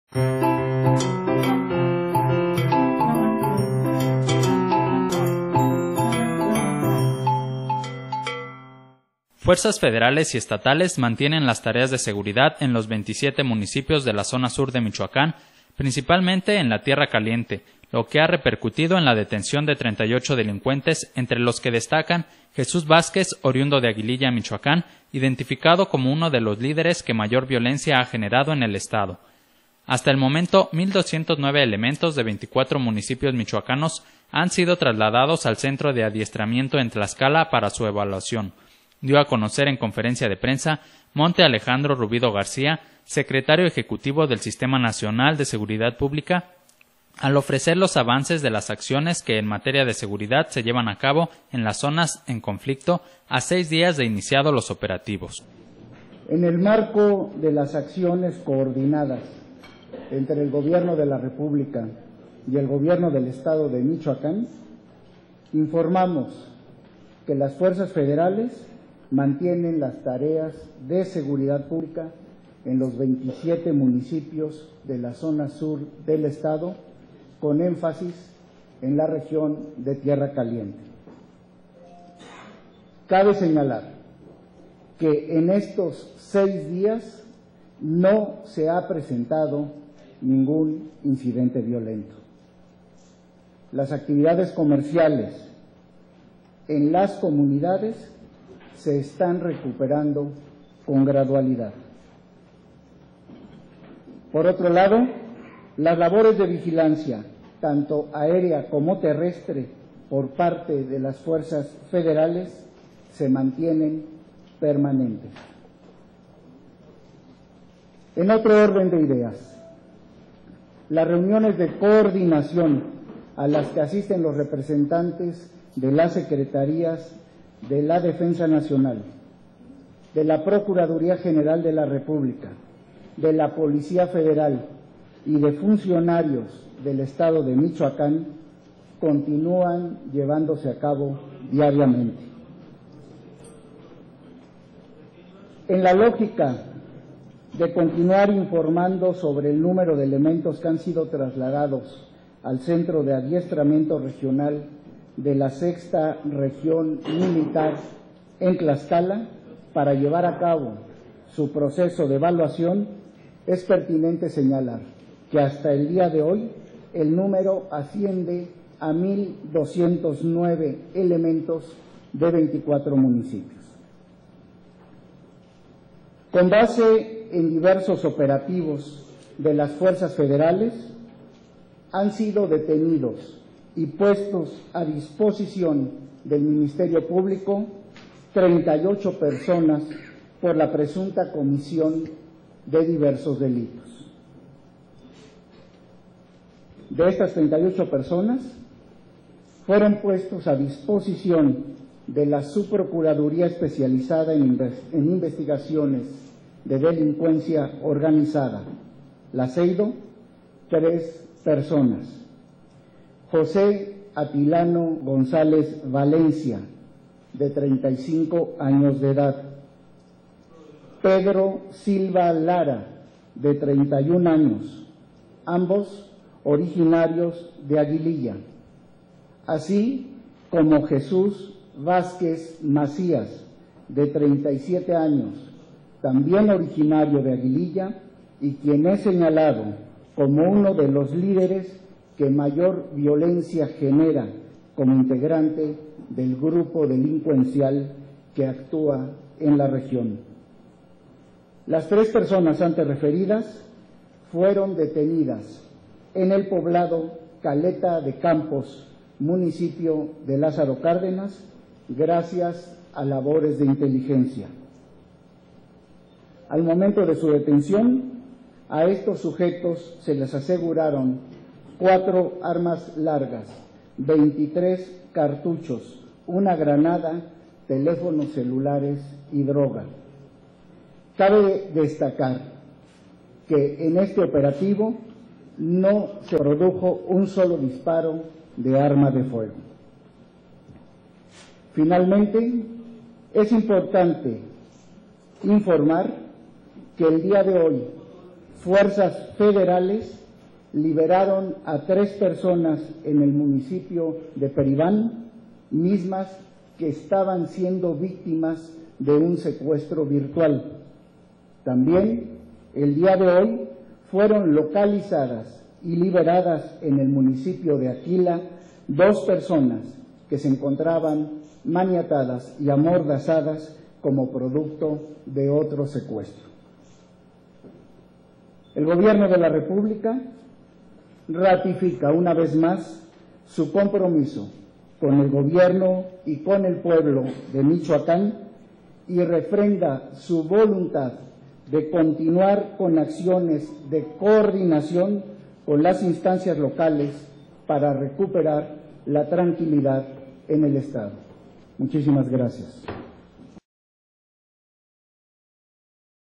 Van 38 detenidos en acciones de fuerzas federales
Fuerzas Federales y estales  mantienen las tareas de seguridad en los 27 municipios de la zona sur de Michoacán, principalmente en la Tierra Caliente, lo que ha repercutido en la detención de 38 delincuentes, entre los que  destacan Jesús Vázquez,  oriundo de Aguililla, Michoacán identificado como uno de los líderes que mayor violencia ha generado en el estado.